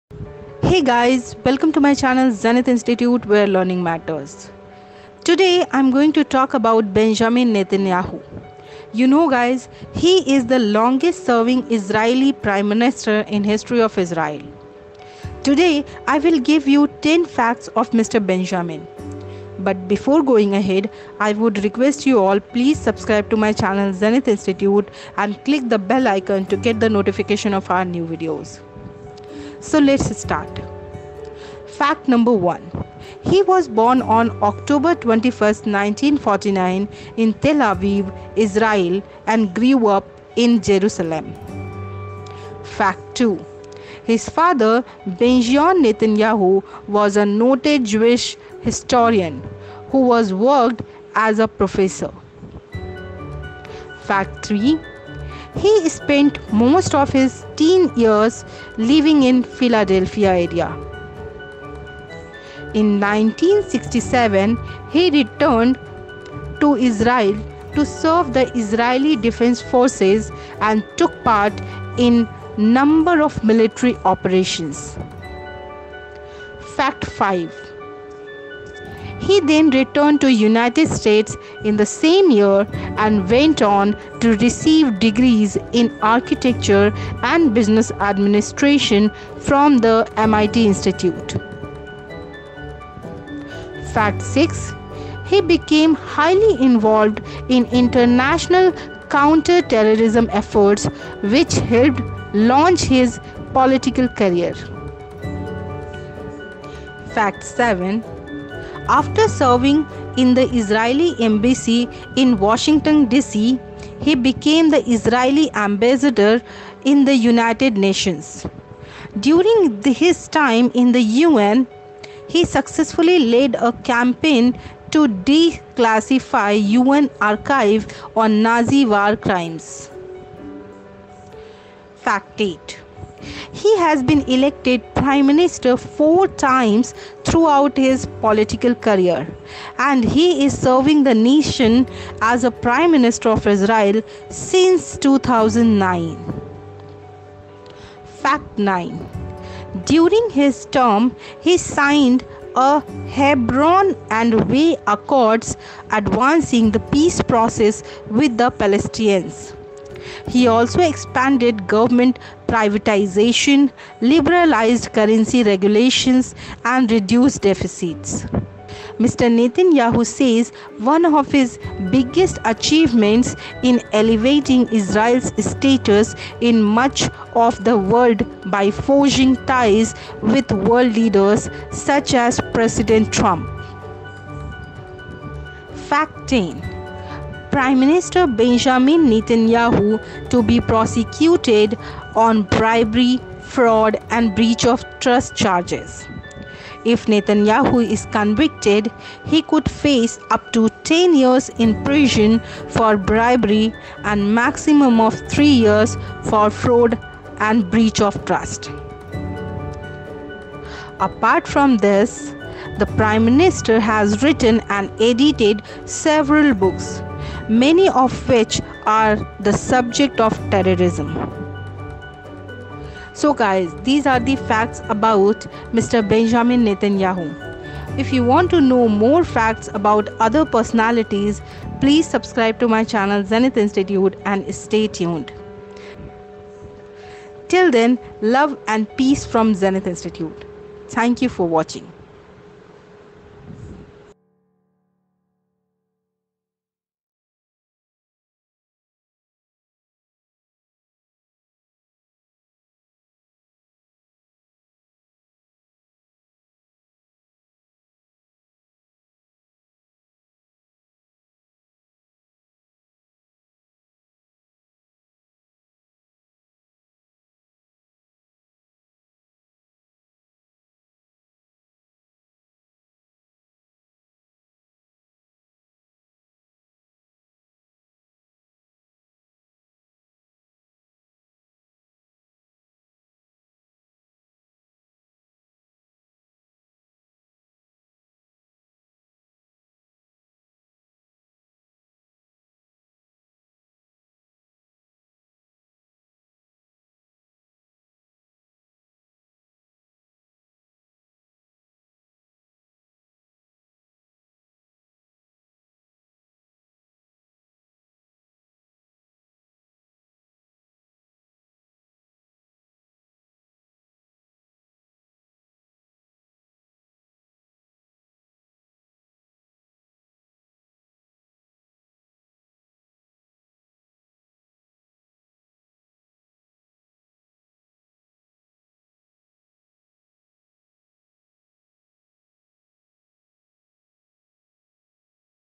Israeli Prime Minister Benjamin Netanyahu - Benjamin Netanyahu Documentary
In his 15 years as Israel's prime minister, Benjamin Netanyahu has set two landmarks by becoming both its longest-serving leader - and its first to face criminal prosecution while in office. Get the full details in this video.